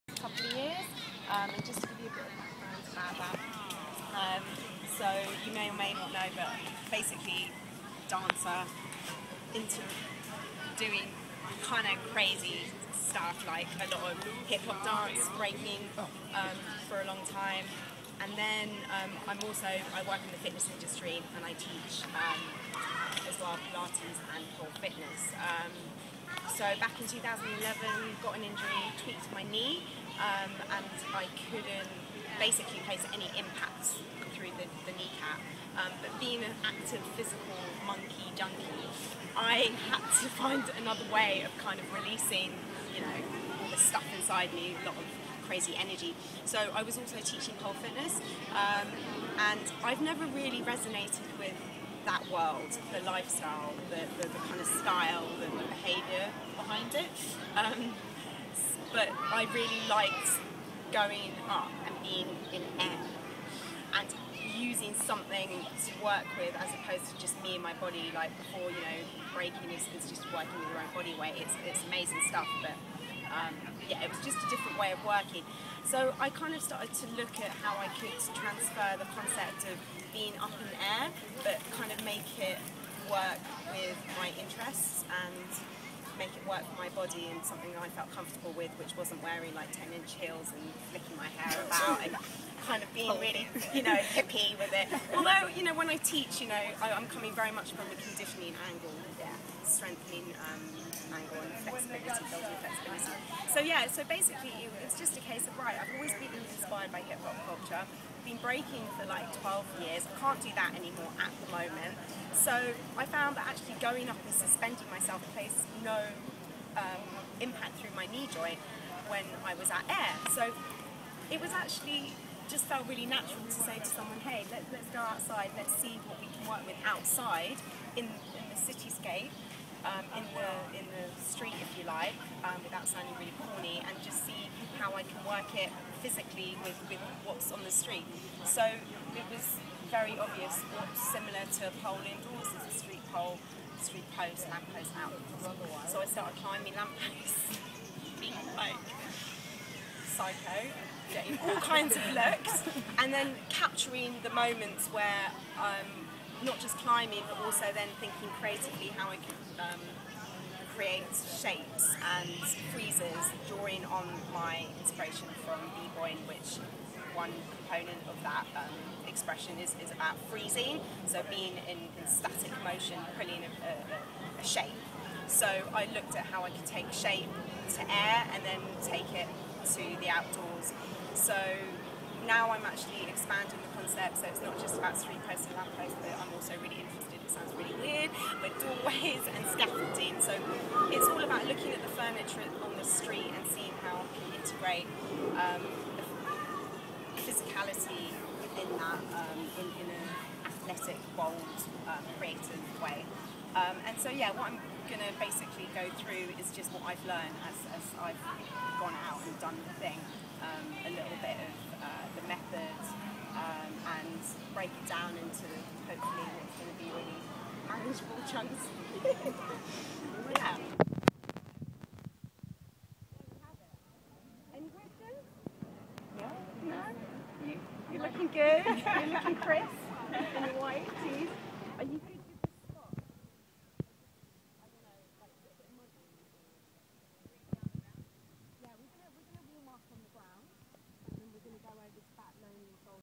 NCG Street Pole Classes in NYC
NCG brings her unique NCG Street Pole Classes to New York City. Find out exactly what it's all about.

This is a taster for the official video which will be coming out later this month!